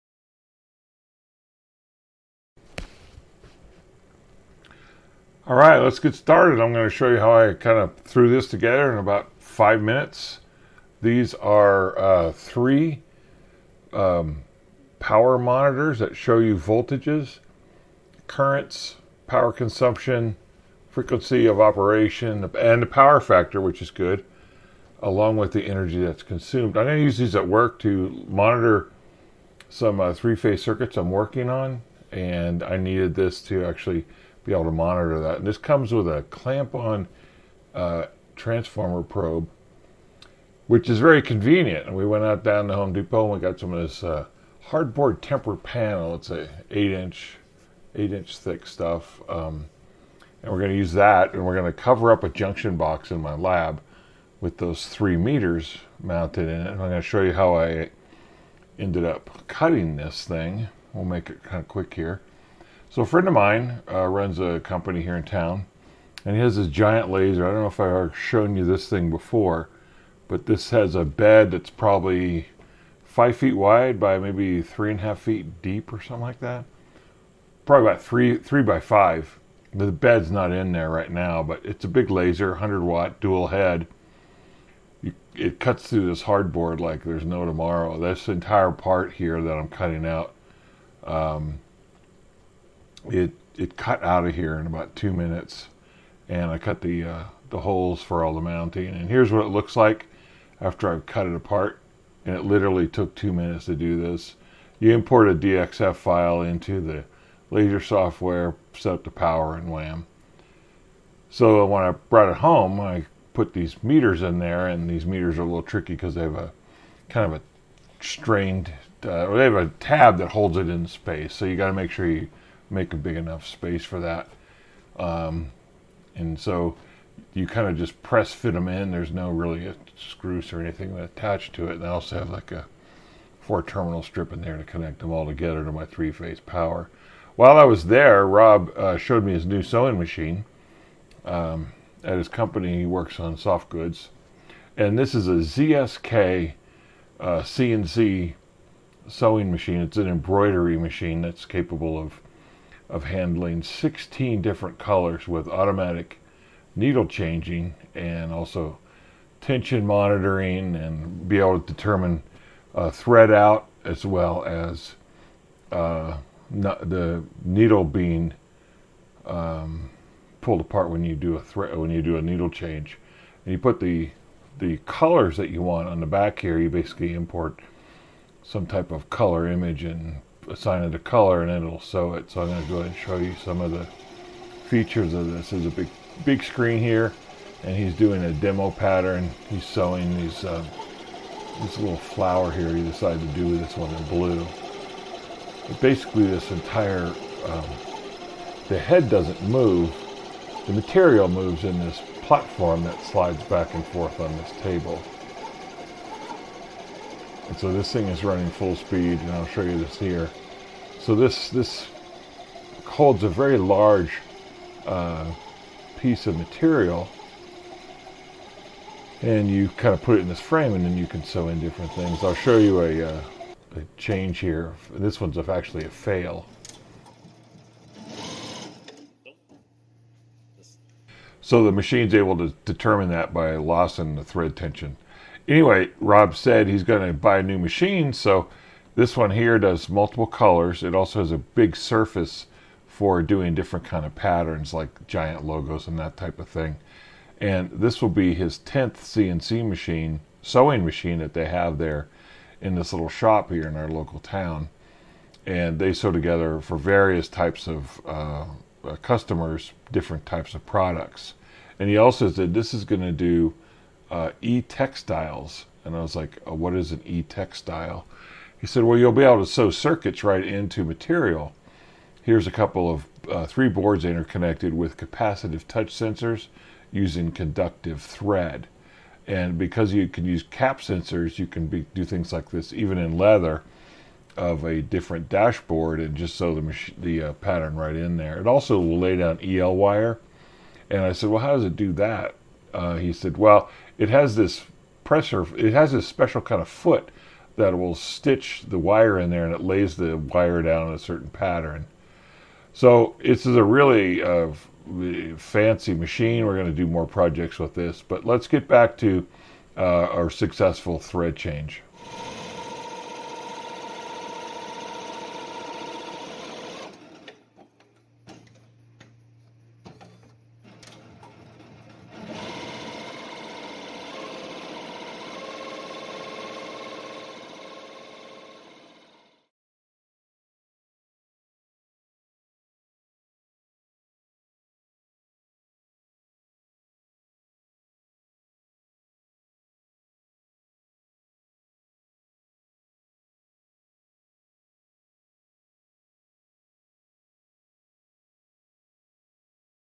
CrocSees the ZSK
Here I use hardboard to mount 3 energy monitors for 3-phase 208V-40A power monitor junction box cover.

CrocSee AC 80-260V 100A CRS-022B LCD Display Digital Current Voltage Power Energy Frequency Power Factor Multimeter Ammeter Voltmeter with 100A Split Core Current Transformer

Rob then shows me new ZSK CNC sewing machine, which is AWESOME! Actually its called an E-Textile Embroidery Machine.